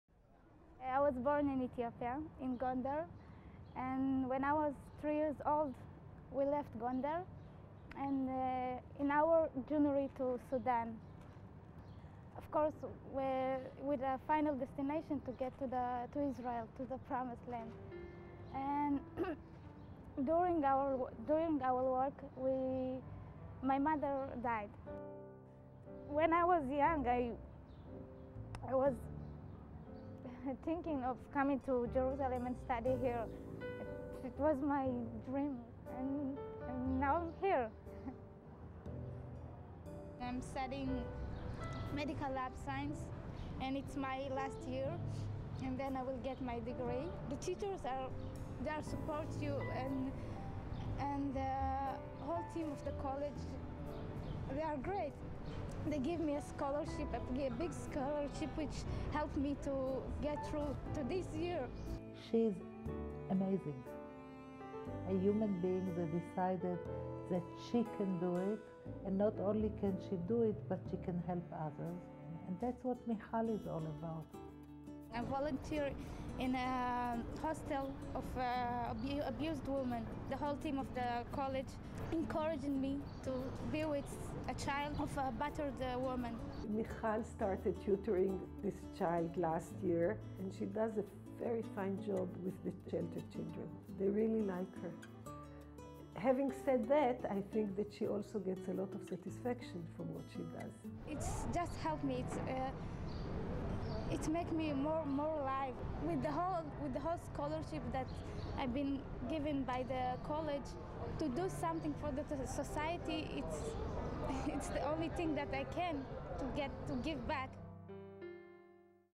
Hadassah College; Michal's story
One Hadassah College graduates story. Emigrating from Ethiopia, losing her mother, then finding HCJ has changed this young womans life and given her hope for the future.